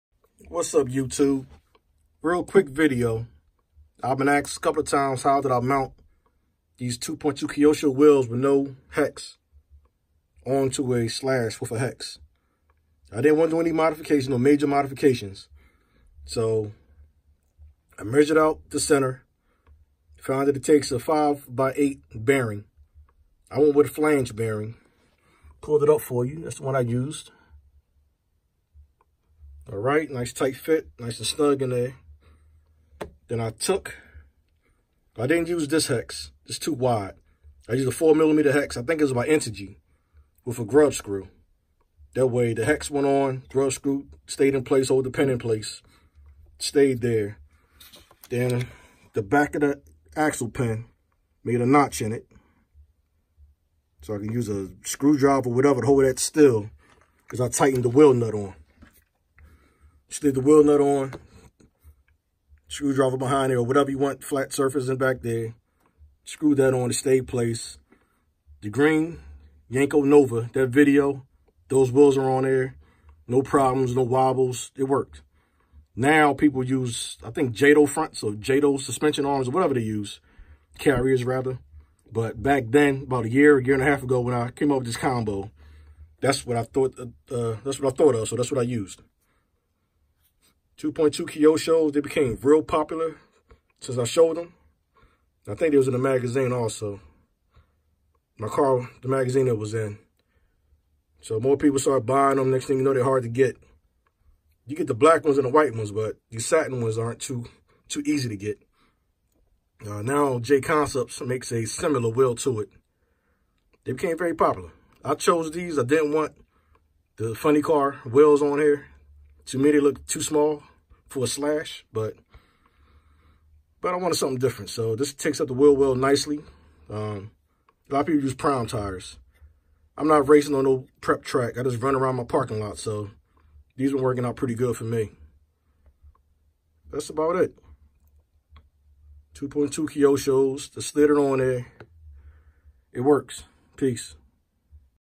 Mounting Kyosho Wheels on a Slash 2wd
As I said in the video, I’ve since seen guys using Jato carriers and even cuts made in the wheel to slide it on the pin without using a hex adapter...the way Bandit stock wheels mount. But my way worked for me.